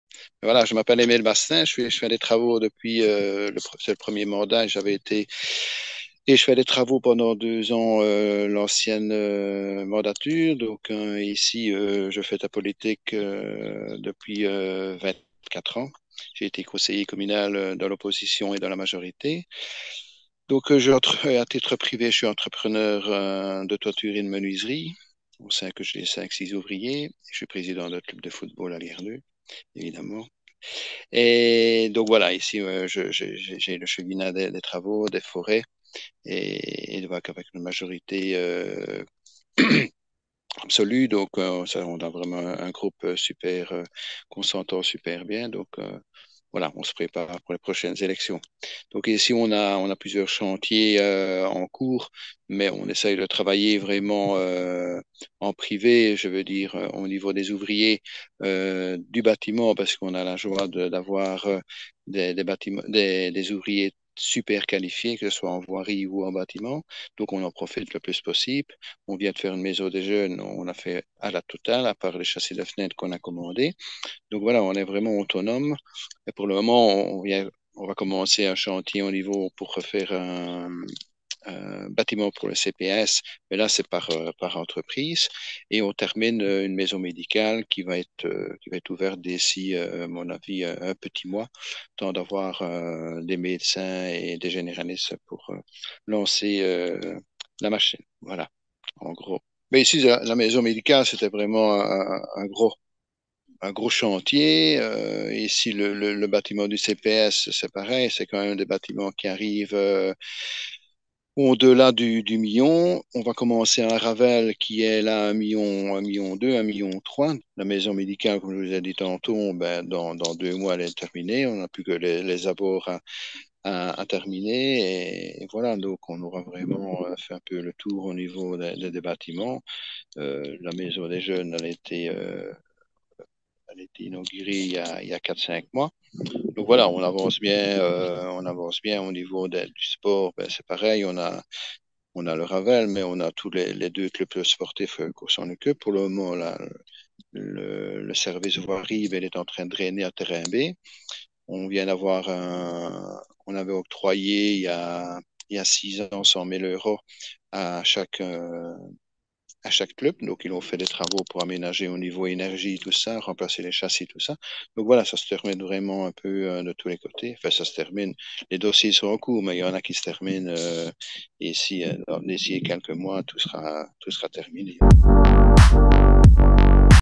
Lierneux : Plusieurs chantiers importants sont en cours.
Emile Bastin : Échevin des Travaux de Lierneux